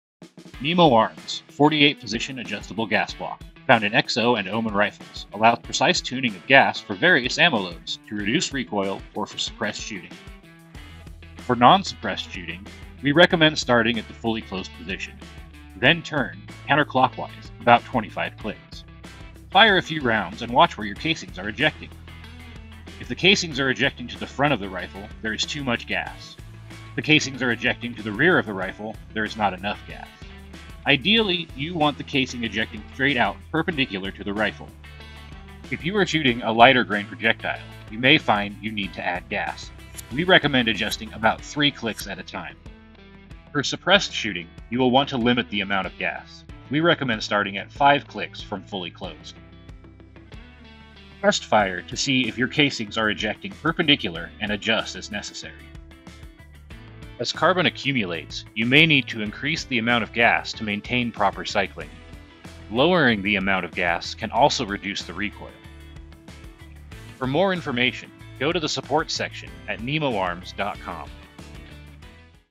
Adjusting the Nemo Arms 48-position Gas Block
This Nemo Arms support video provides information on adjusting our 48-position gas block for unsuppressed and suppressed shooting. The 48-position gas block is found on all XO and Omen rifles after mid-2023.